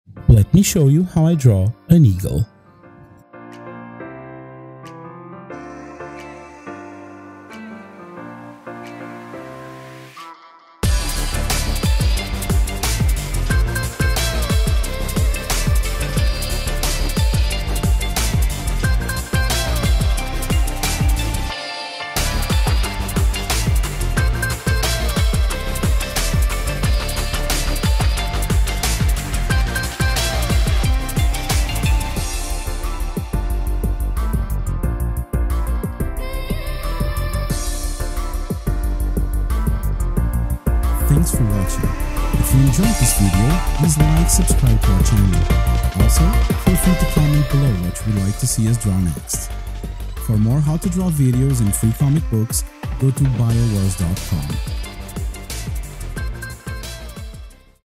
EAGLE DRAWING For Beginners
Dive into the world of artistry with our step-by-step guide on eagle drawing!
Unleash your creativity as we break down the process, from sketch to shading. In this beginner-friendly tutorial on how to draw an eagle, you’ll learn how to:
• Outline the eagle’s body
• Enhance the sketch with thicker lines
• Add details to your eagle drawing to bring it to life
• Shade the sketch

For more drawing tutorials, comic book insights, and exciting stories, visit BIOWARS, an online community for comic book fans & creators

Which tutorial would you like us to cover next?
• Something else

Chapters:
• 00:00 Intro
• 00:05 Drawing Basic Shapes
• 00:11 Adding Thicker Lines
• 00:23 Shading (filling in the object)
• 00:35 Adding Final Details
• 00:41 Outro

Find this simple guide on how to draw an eagle in our beginner-friendly illustrated tutorial on BIOWARS